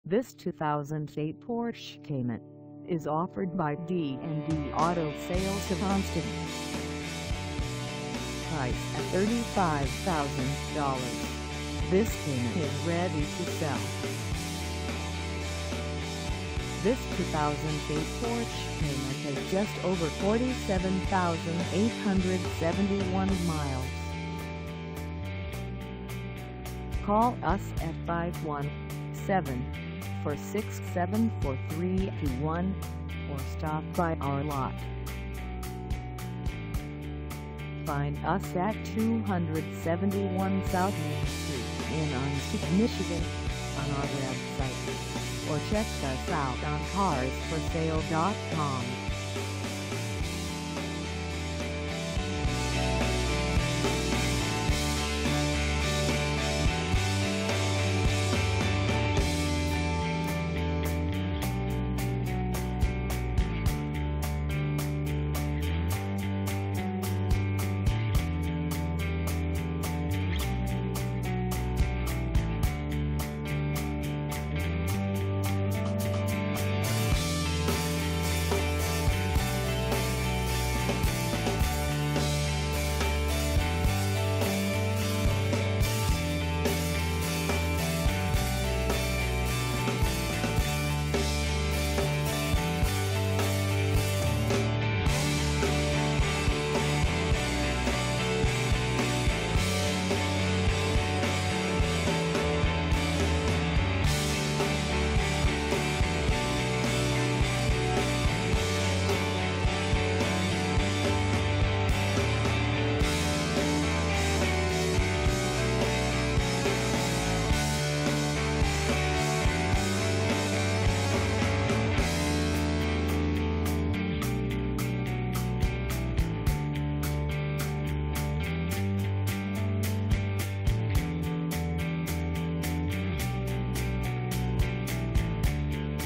2008 Porsche Cayman S 2dr Coupe for sale in Onsted, MI 49265
This 2008 Porsche Cayman S 2dr Coupe is for sale in Onsted, MI 49265 at D and D Auto Sales Of Onsted.

Engine: 3.4L H6
Transmission: Automatic 5-Speed
Color: Red
Mileage: 47871
Contact D and D Auto Sales Of Onsted 271 S Main St Onsted, MI 49265 to test drive this 2008 Porsche Cayman S 2dr Coupe today.